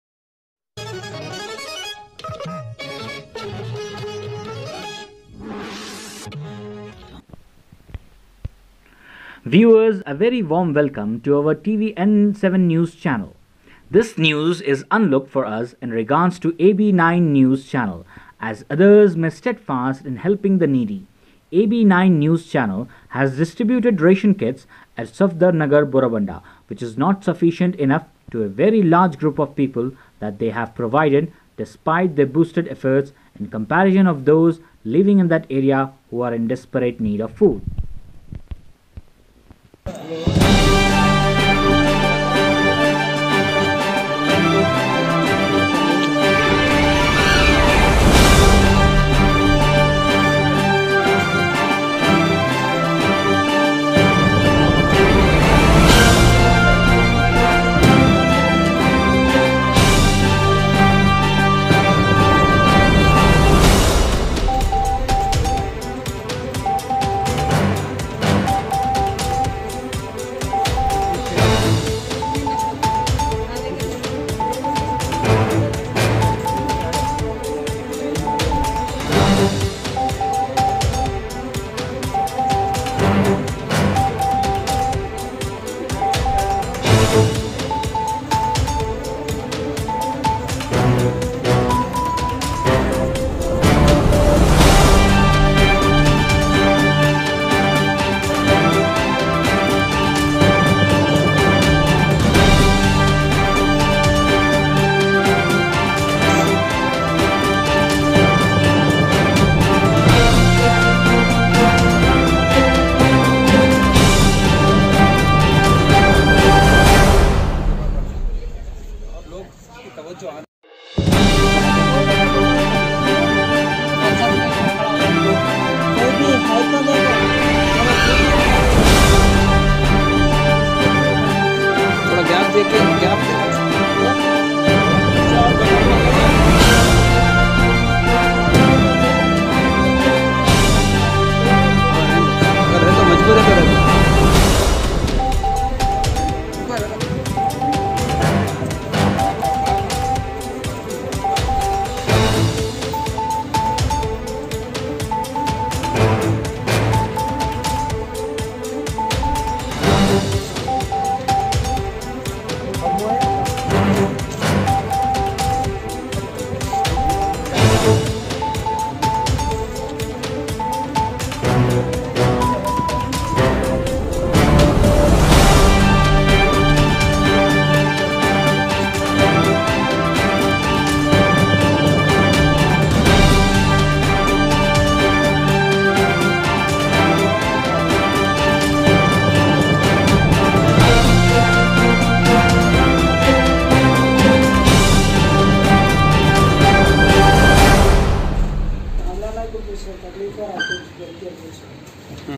This news is unlooked for us inregards to AB9 NEWS Channel as others may steadfast in helping the n
Viewers a Very Warm Welcome to our TVN7 News Channel

This news is unlooked for us inregards to AB9 NEWS Channel as others may steadfast in helping the needy. AB9 NEWS Channel has distributed ration Kits at Safdarnagar, Borabonda, which is not sufficient enough to a very large group of people that they have provided despite their boosted efforts in comparison of those living in that area who are in desparate need of food.